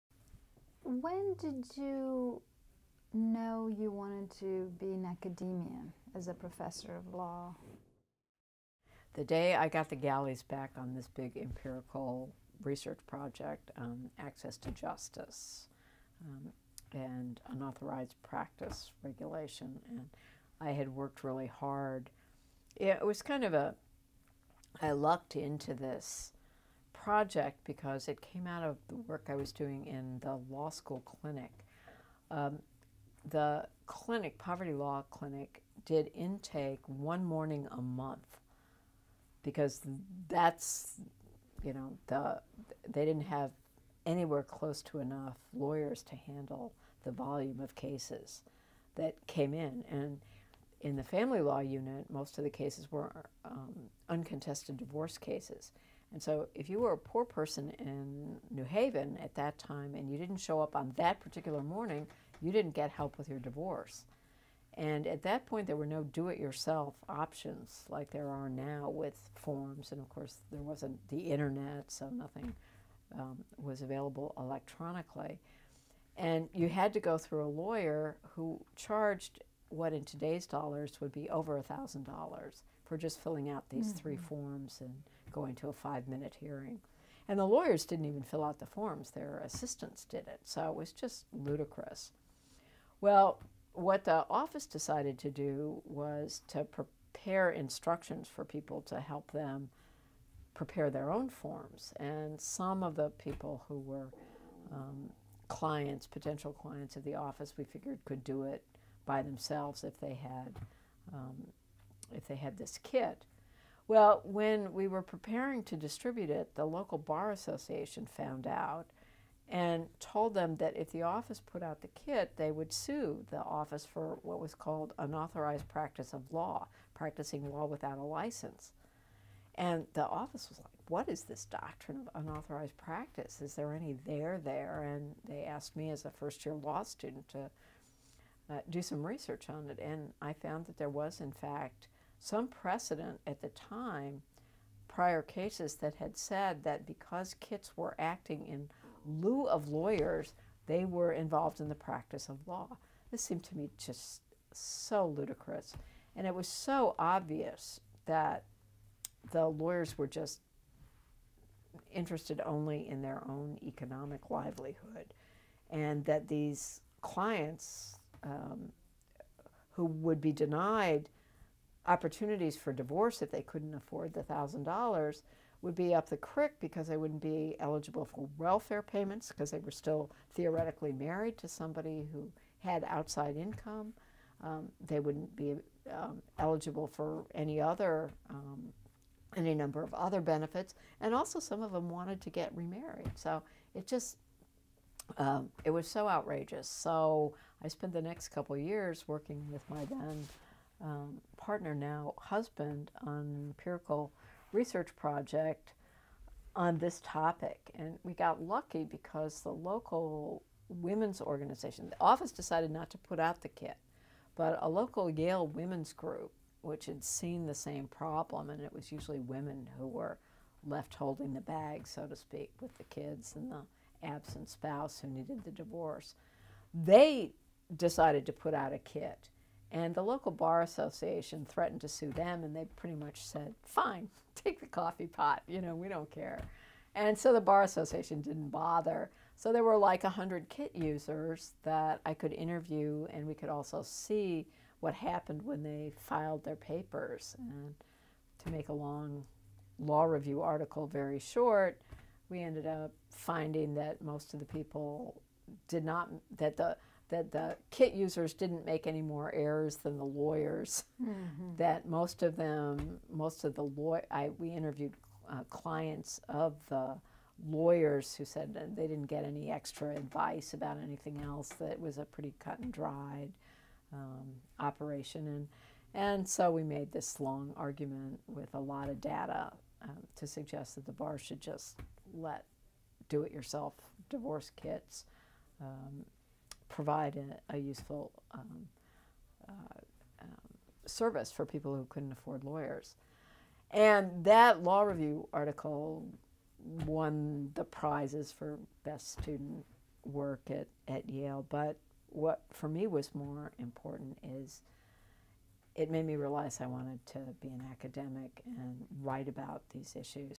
Deborah Rhode: Decision to Become a Law Professor
In this excerpt from a 2018 oral history for the Stanford Faculty Oral History Project, Professor of Law Deborah L. Rhode describes how a research project with a poverty law clinic on the efficacy of do-it-yourself divorce kits, which provided poor women with broader access to the legal system, led to her decision to pursue an academic law career.

For the full interview transcript and recording, please visit our